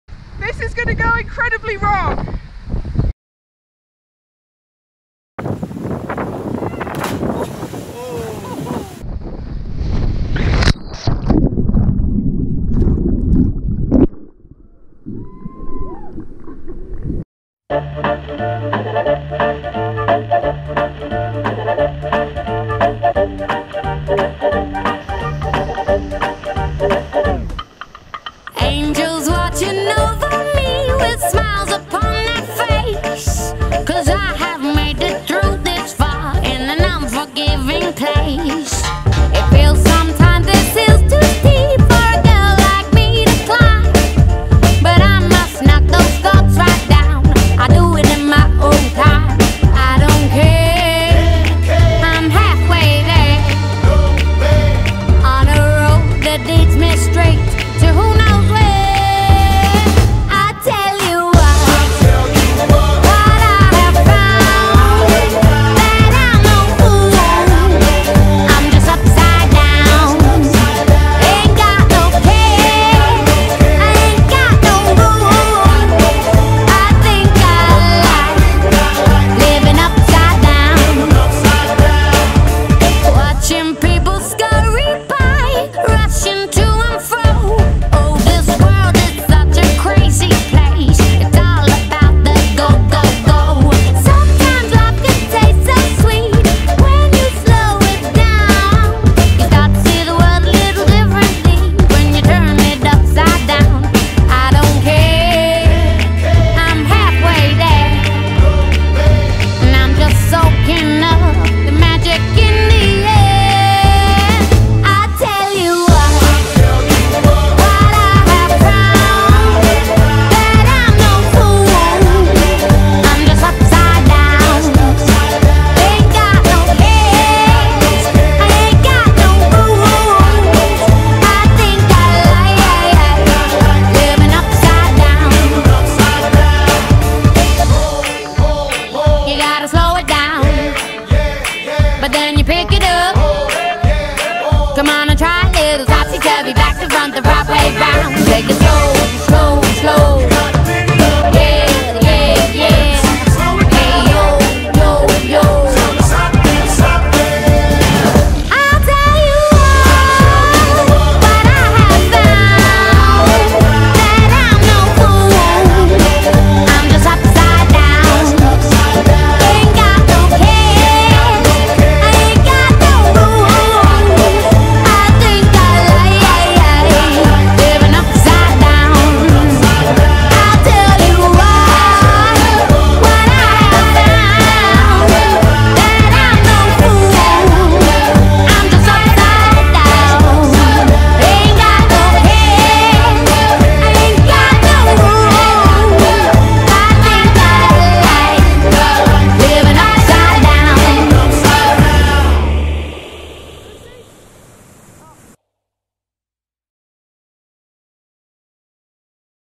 DUCC Lakes 2019
Weekend away in the Lake District with Durham Uni Canoe Club. Lots of swims, good company and many smiles made it a great time!